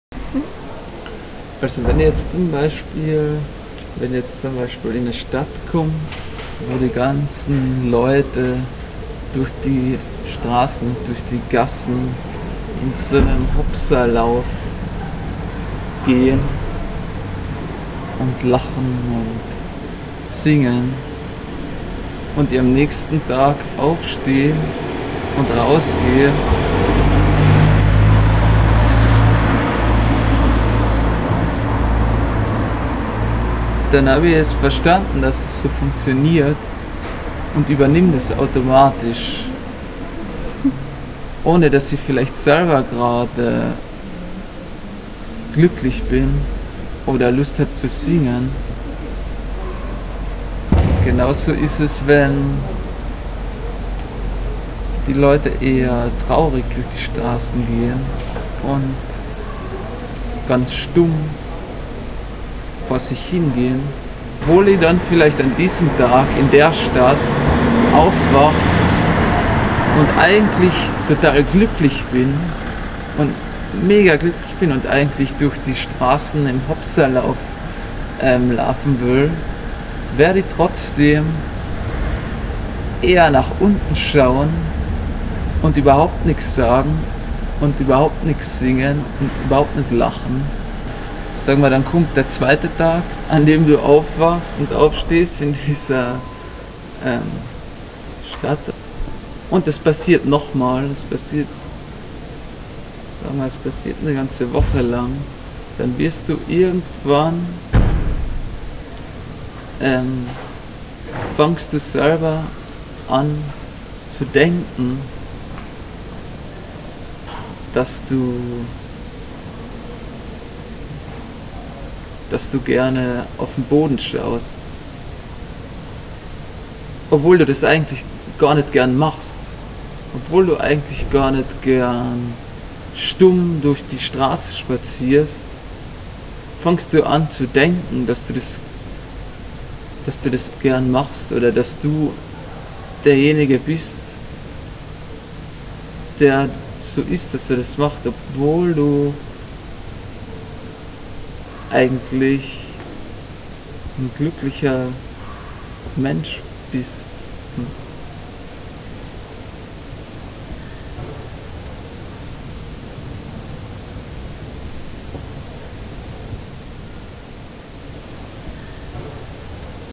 ARTMAGAZIN amLeben - Gregor Esra Sauer 2/2 (Interview)
Man kennt Gregor Esra Sauer als Musiker der Band "Kartenhauskörper" und als Bildhauer und Maler. Wir haben ihn in Wien getroffen zu einem kurzen Interview (2/2)